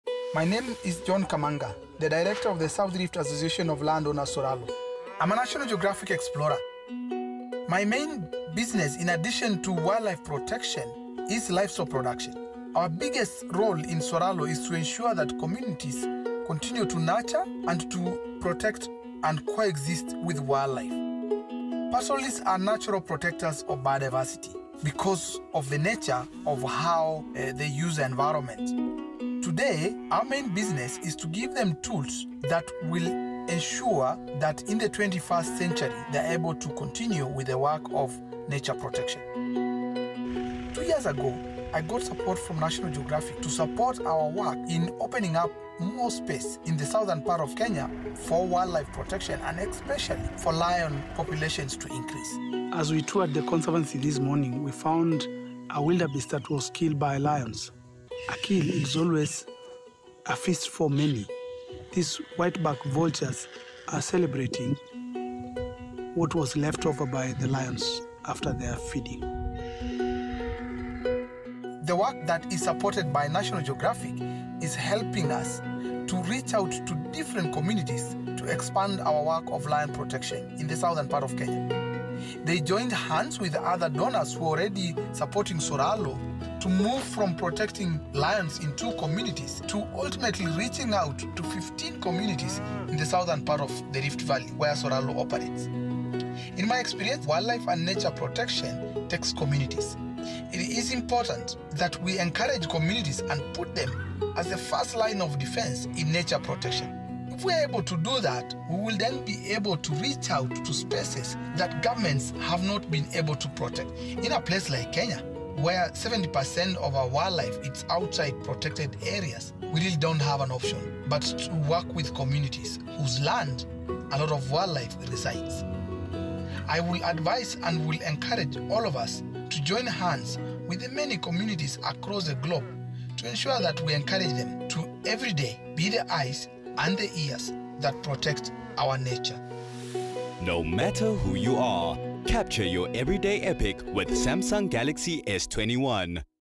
John Kamanga talks communities and wildlife protection
National Geographic Explorer John Kamanga works with communities to act as the first line of defense in nature protection. He is based in Kenya, where 70% of wildlife is outside protected areas. Watch him at work in this short clip, shot on a Samsung Mobile Galaxy S21 Ultra 5G